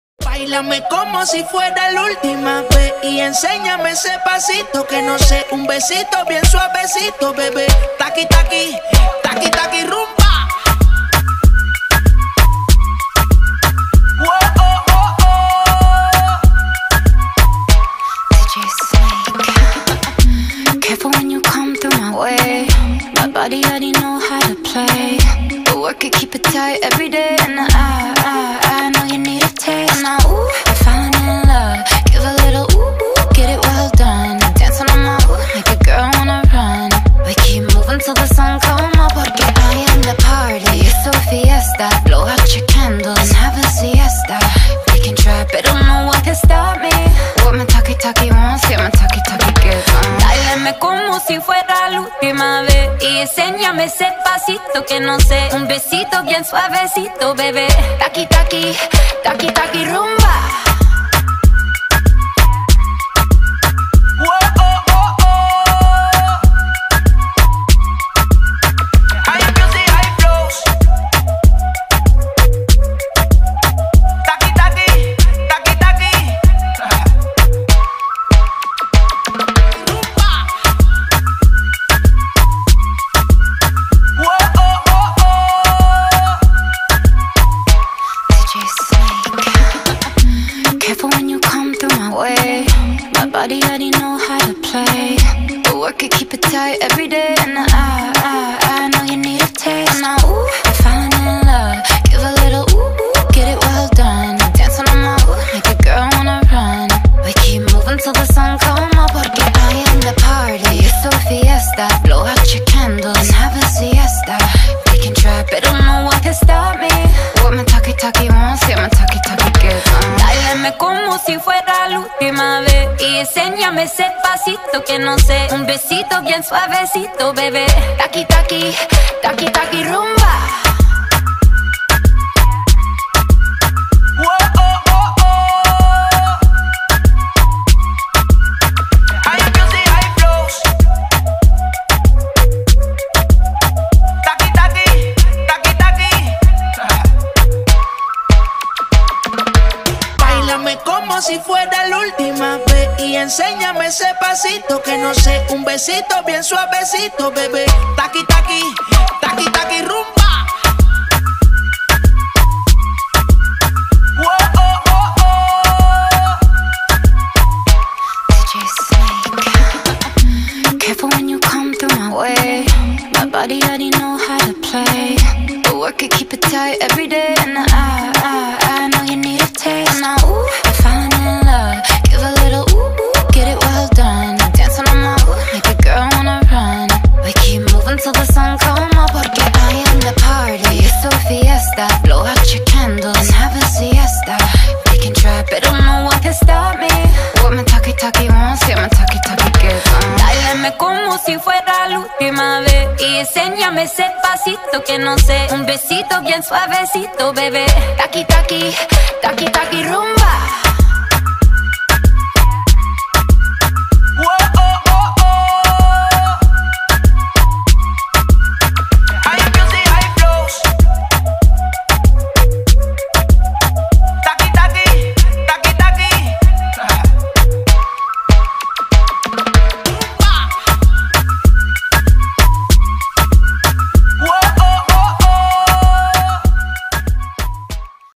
Bike Cleaning After Heavy Rain Season In Karachi | Yamaha YBR 125-G | Asif Mir Vlogs
Bike Cleaning After Heavy Rain Season In Karachi | Yamaha YBR 125-G | Asif Mir Vlogs | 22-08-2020